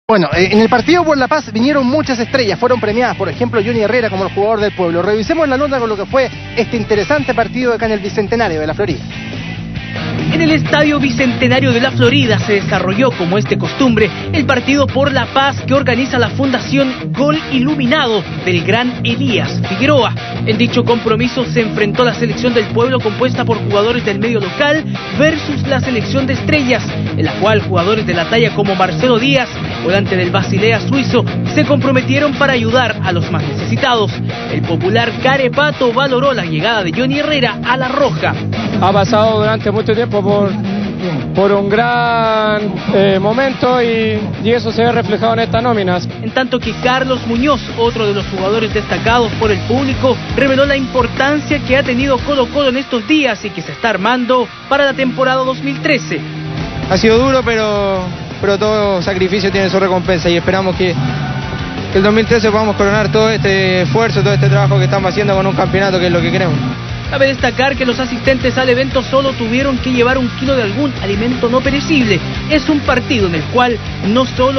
La Selección del Pueblo superó a las Estrellas en "Partido por la Paz" | 29.12.2012
La Selección del Pueblo se impuso al Equipo de las Estrellas por 4-2 en el Estadio Bicentenario de La Florida, en una nueva edición del "Partido Por La Paz", evento organizado por la Fundación Gol Iluminado, que lidera Elías Figueroa.